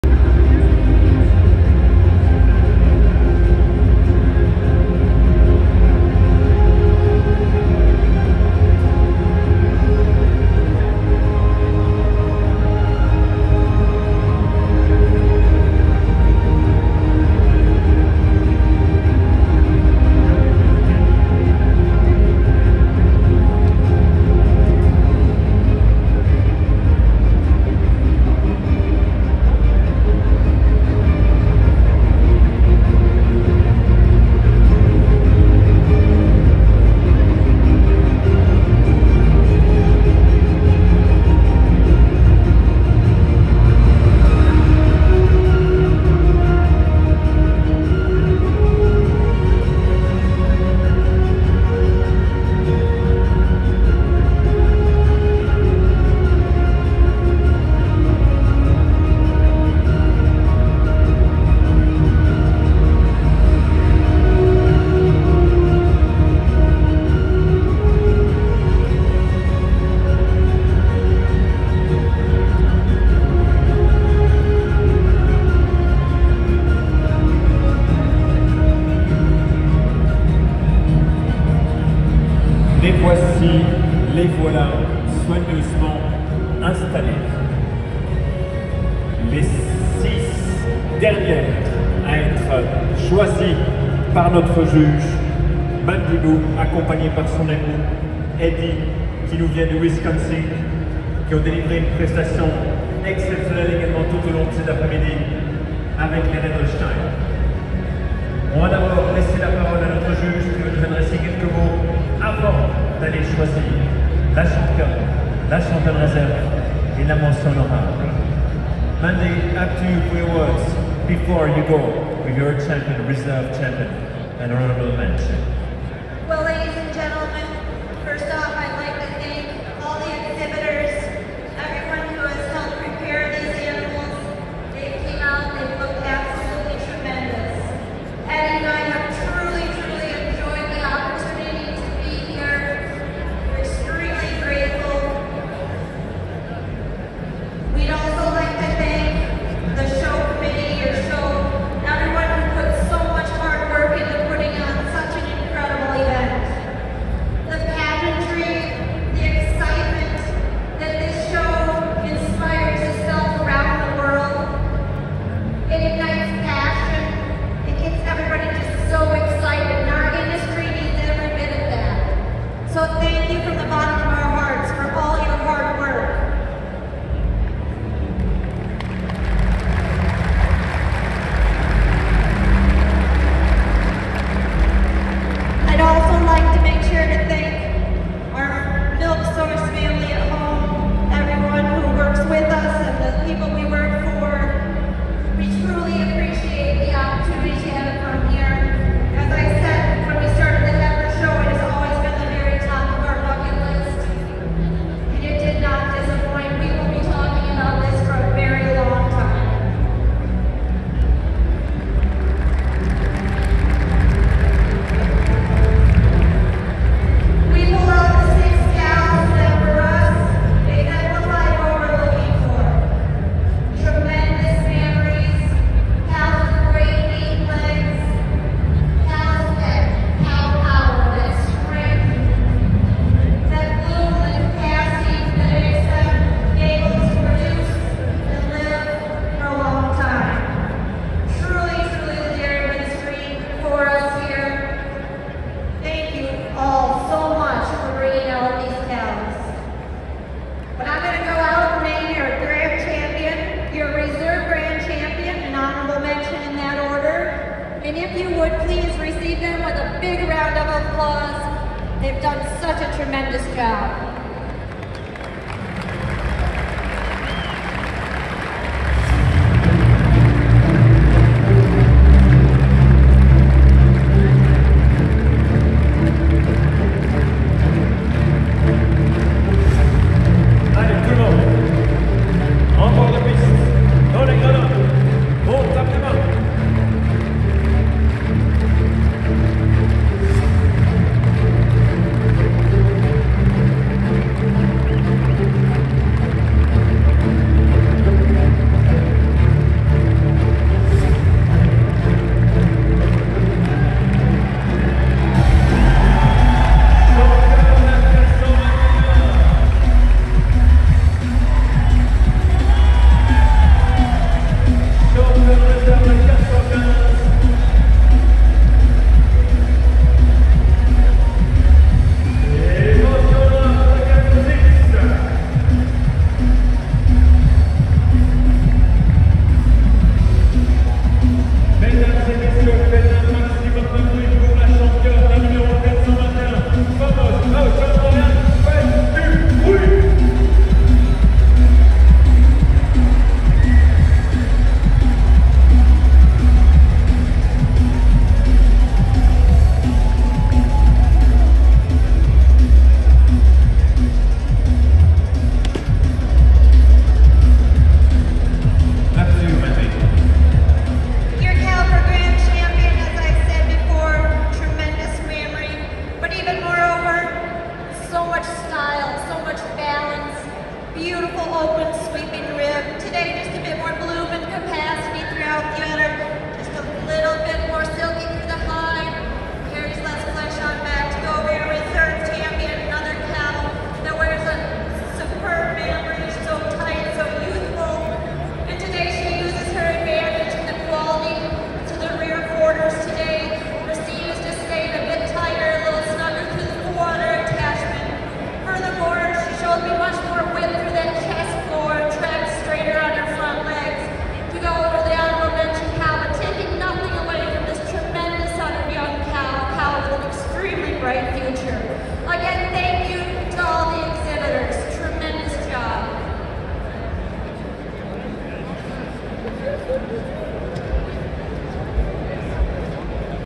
Swiss Expo 2024 Grand Champion RH
Swiss Expo 2024: Hier sehen Sie die Wahl der Grand Champion Red Holstein.